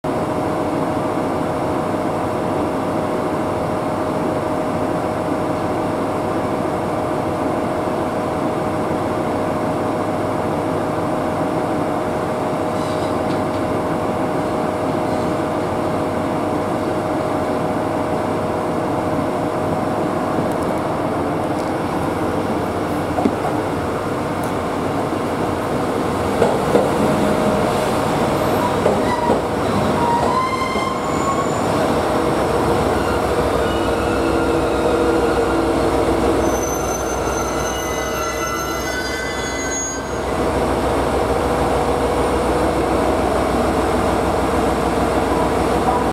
2023.10.14 JR板柳駅 五能線 弘前行き到着【GV-E400系・2835D】 2023.10.14
始発：深浦（15時12分）